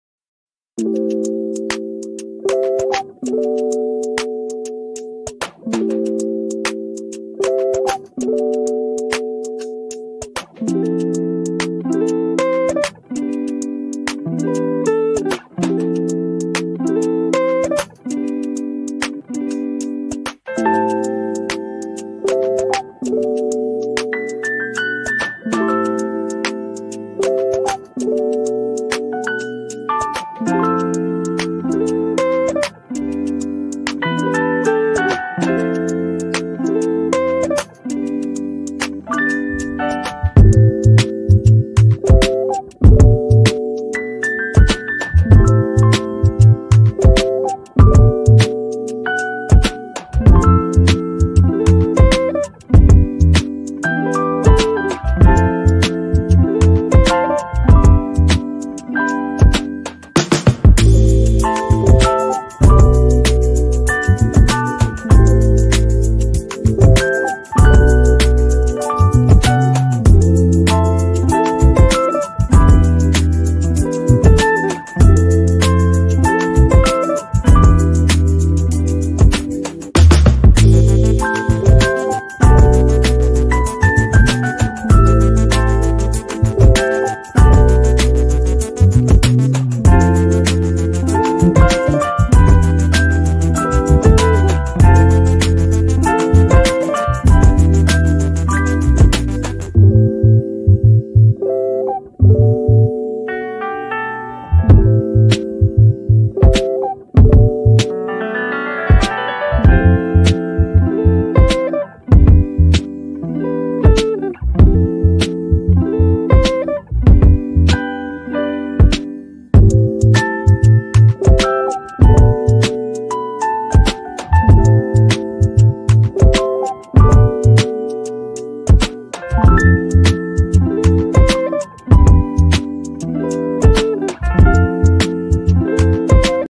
Green Vegetable Upma
Quick and healthy vegetable upma recipe for breakfast.